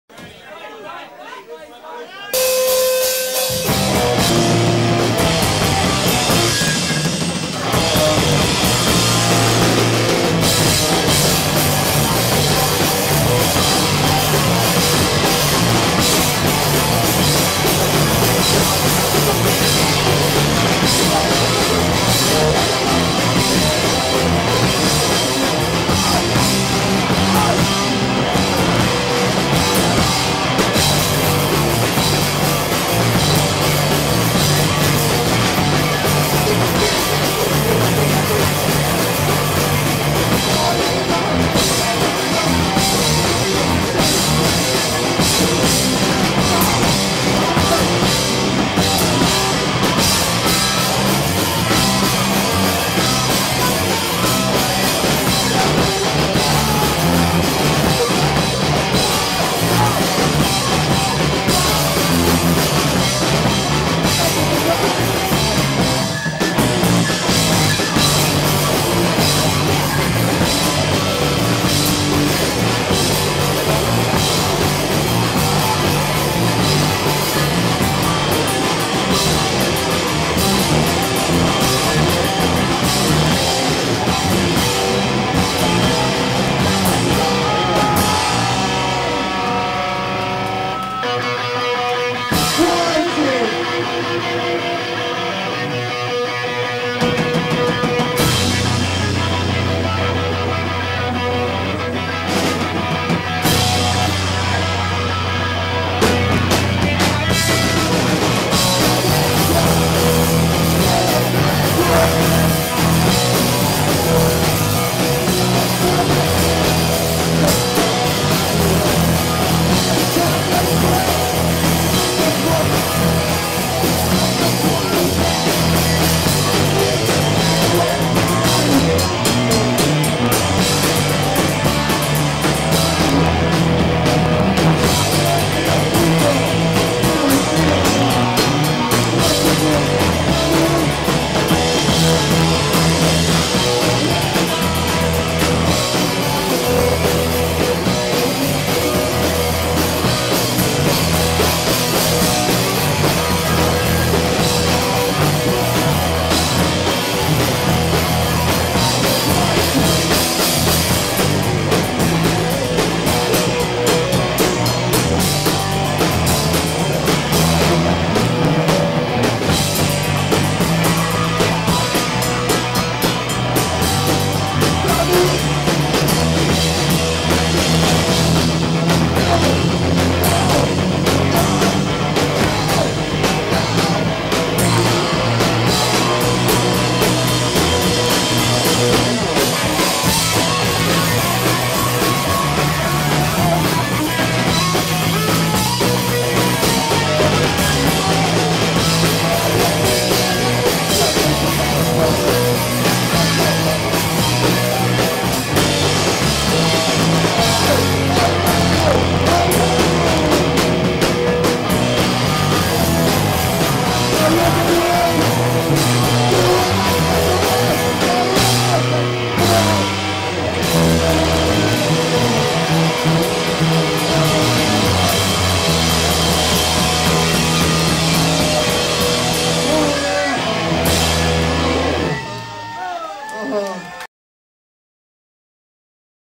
CAREER SUICIDE part 9 (death house) 2011
CAREER SUICIDE à la DEATH HOUSE (Montreal) vendredi 22 avril.

avec crude genes / POISON IDEA COVER / SUBLIMINAL